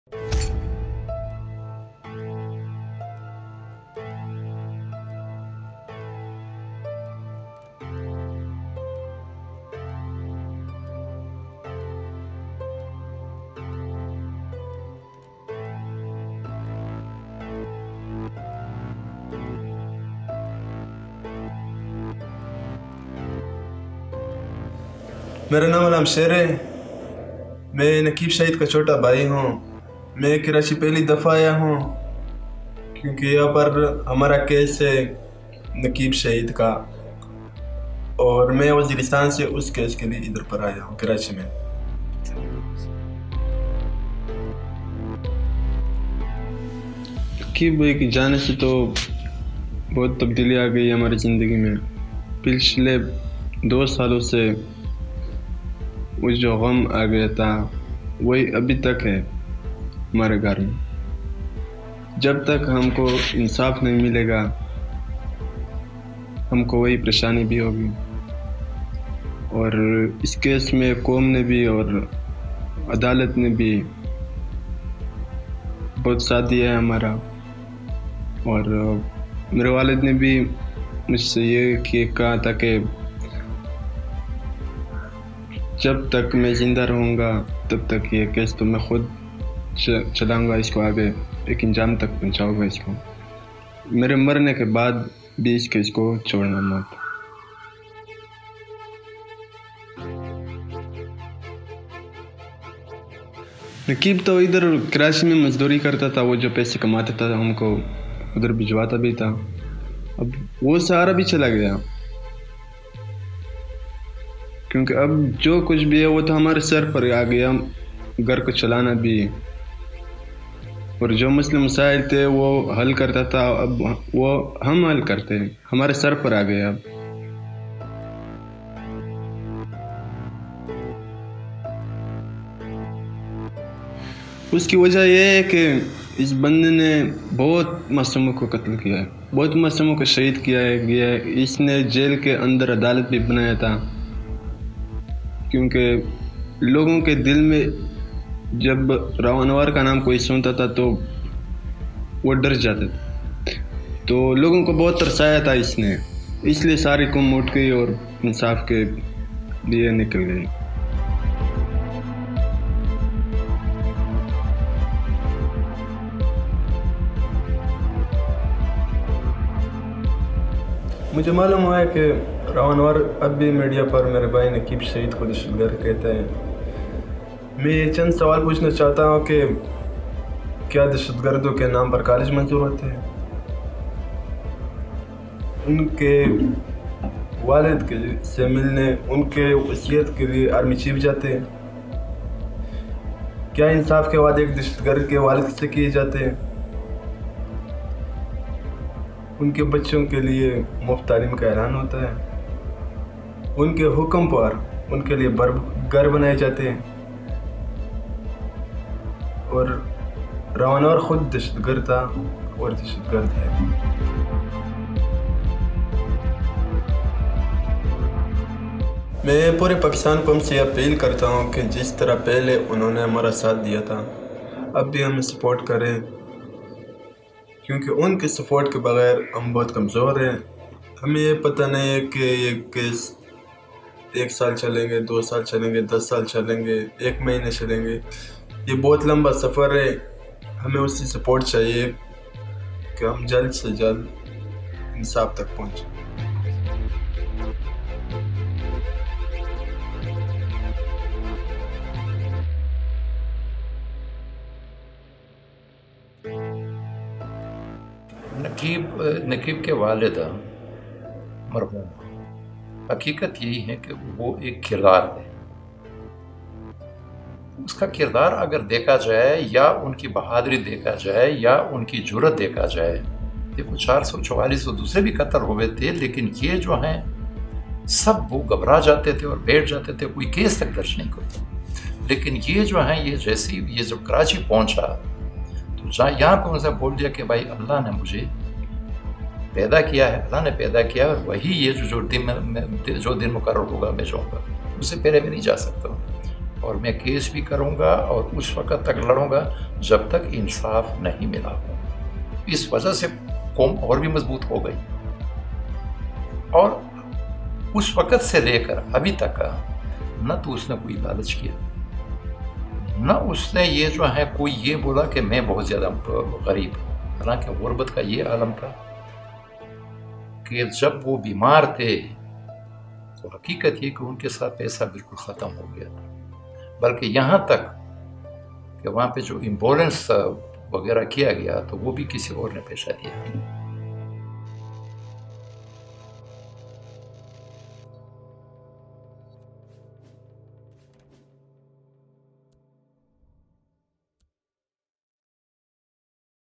Will There Ever Be JusticeForNaqeebullah?
After the passing of Naqeebullah's father, the legal battle against Rao Anwar has now become the responsibility of his 19-year-old brother, Alam Sher Mehsud. TeamSoch spoke to him.